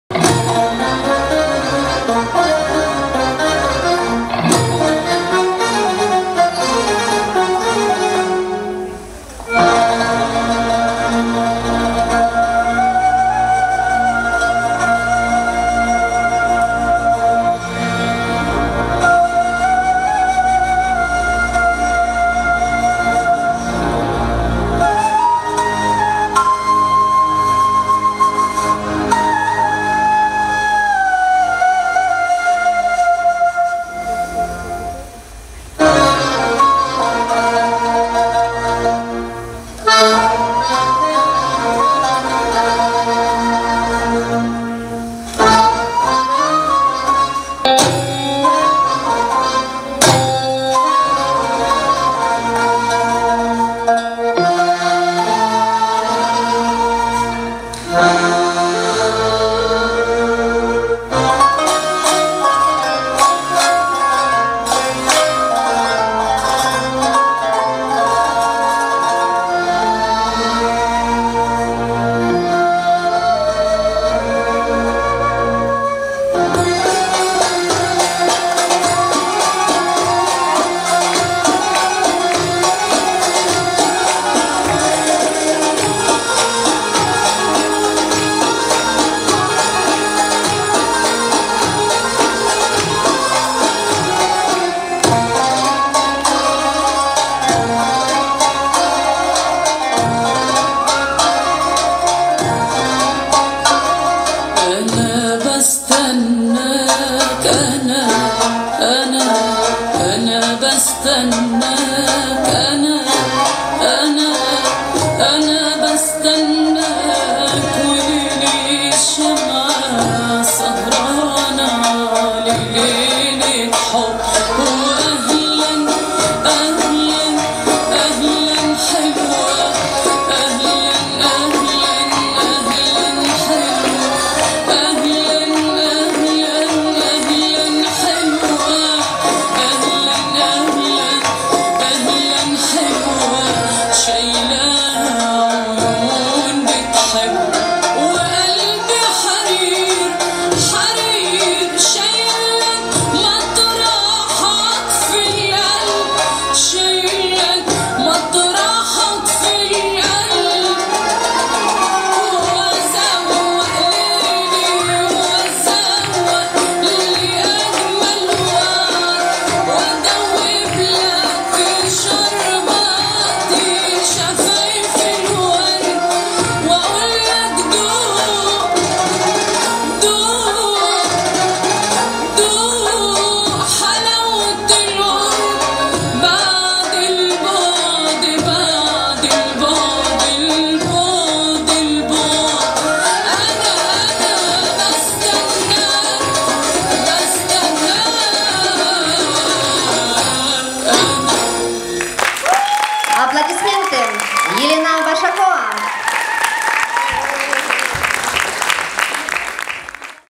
Bashakova Elena (Kiev summer rest'n'fest 2015)
06 Bashakova Elena
gala show of Kiev summer rest'n'fest 2015